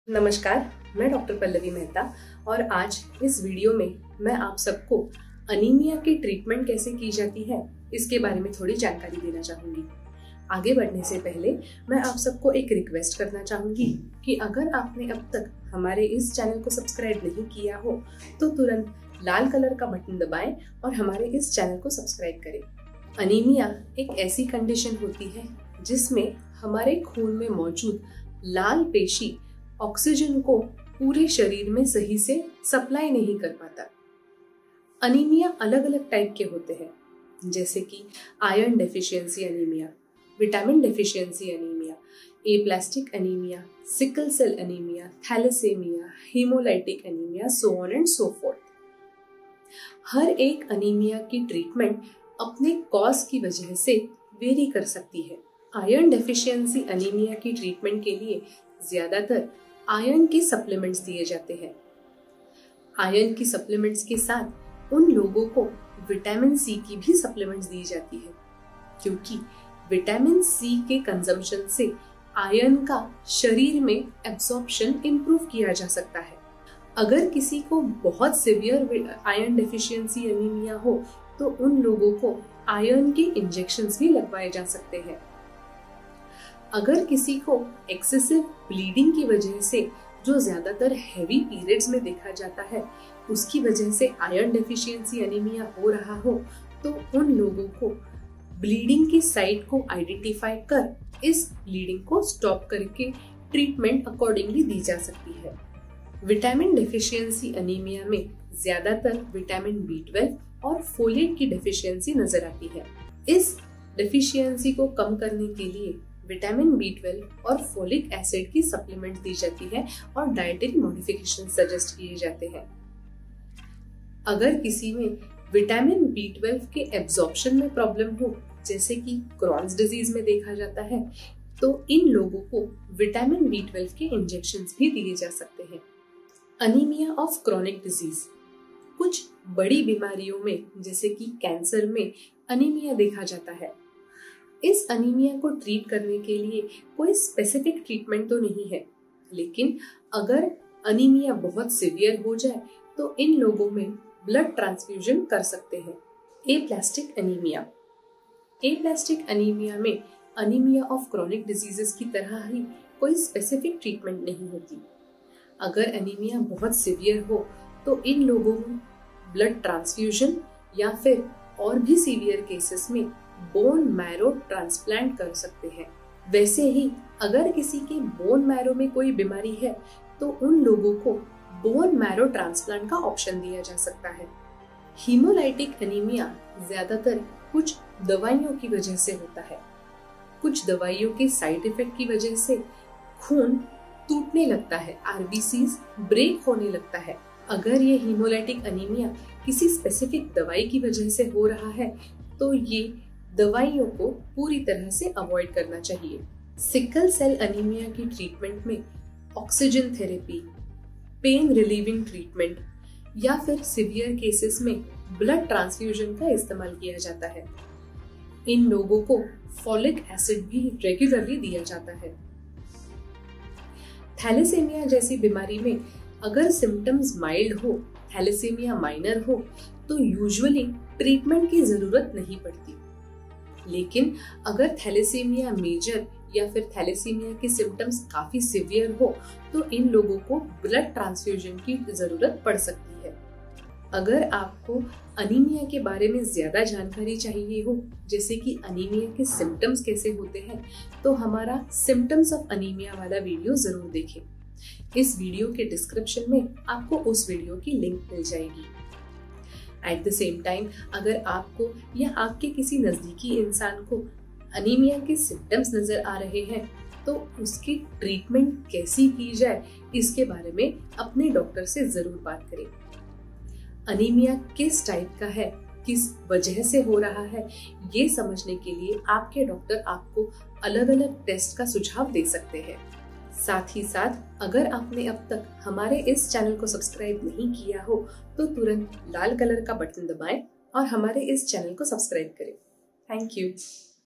एनीमिया को दूर करने के उपाय - Treatment For Anemia in Hindi by Dr. Pallavi M
एनीमिया को दूर करने के उपाय. How to treat Anemia in Hindi. Medical & home treatments & care for anemia patients. Learn the right treatment & care for anemic patients by Dr. Pallavi.